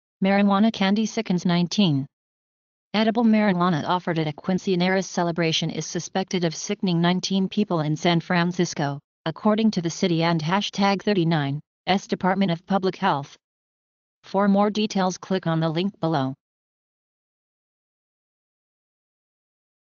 Marijuana candy sickens 19 | By : CNN
Edible marijuana offered at a quinceañera celebration is suspected of sickening 19 people in San Francisco, according to the city&#39;s Department of Public Health.